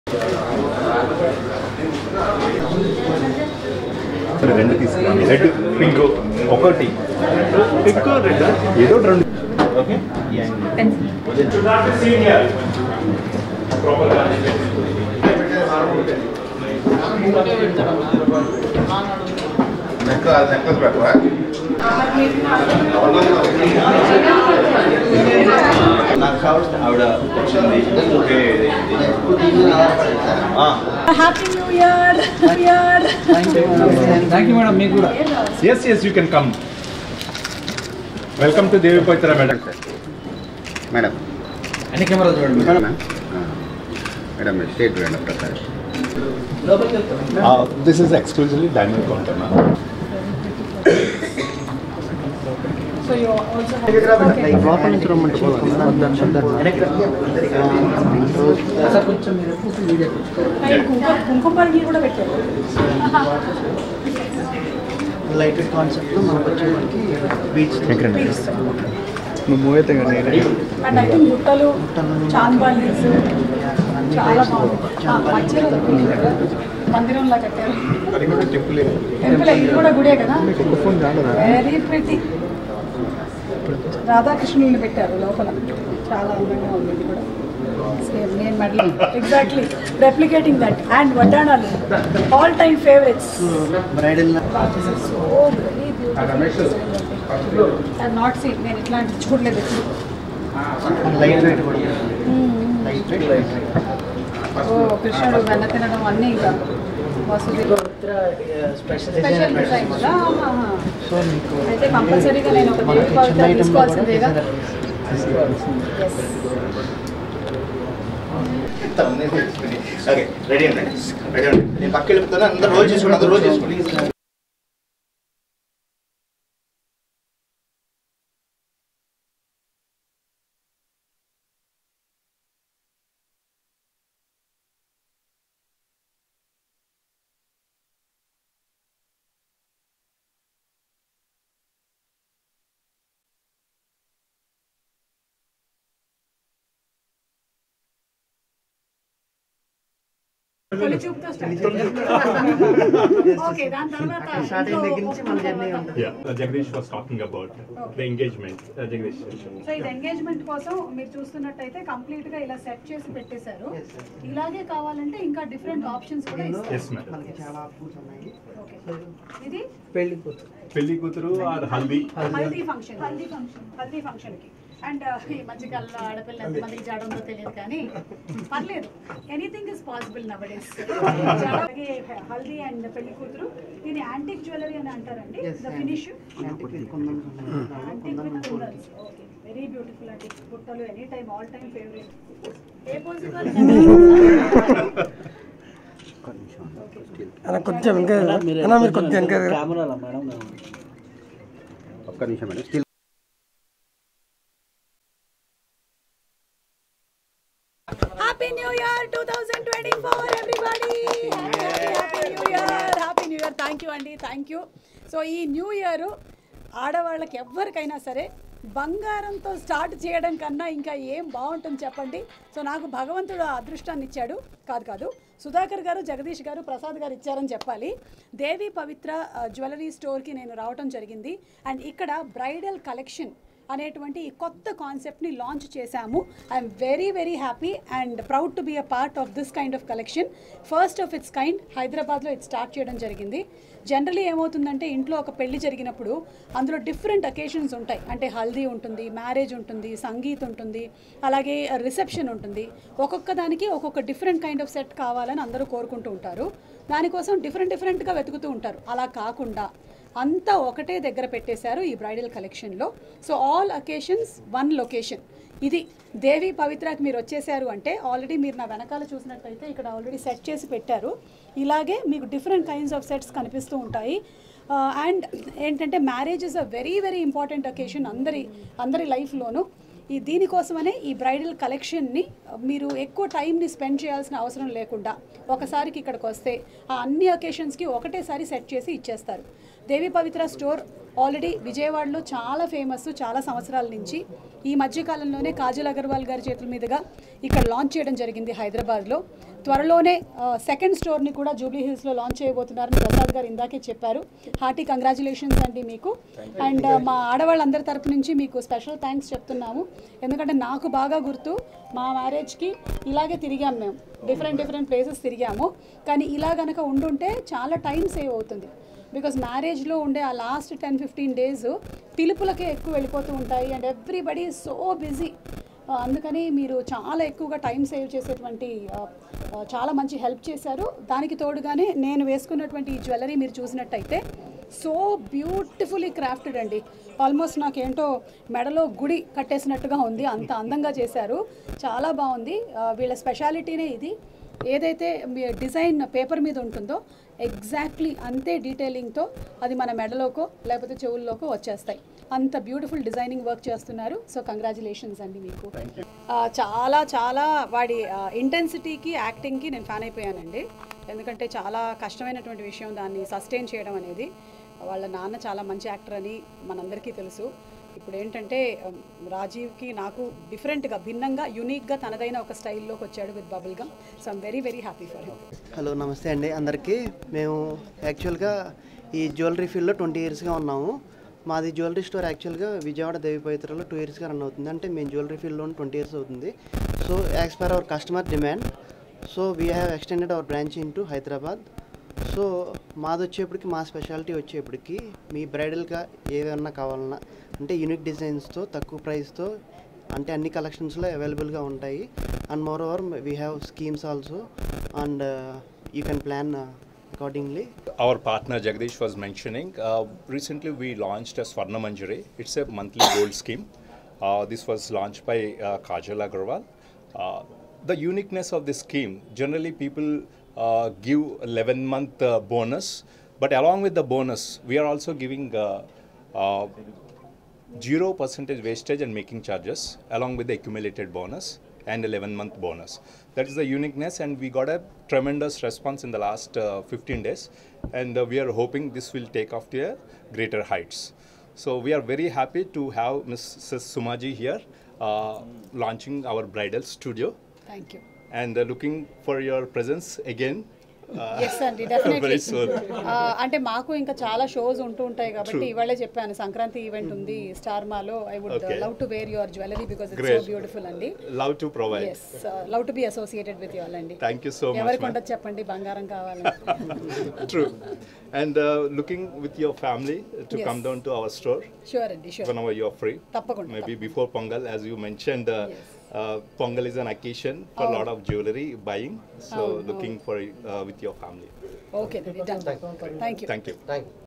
Anchor Suma Launched Bridal Studio in Devi Pavitra Gold & Diamonds at Kukatpally | Silver Screen
Watch : Anchor Suma Launched Bridal Studio in Devi Pavitra Gold & Diamonds at Kukatpally | Silver Screen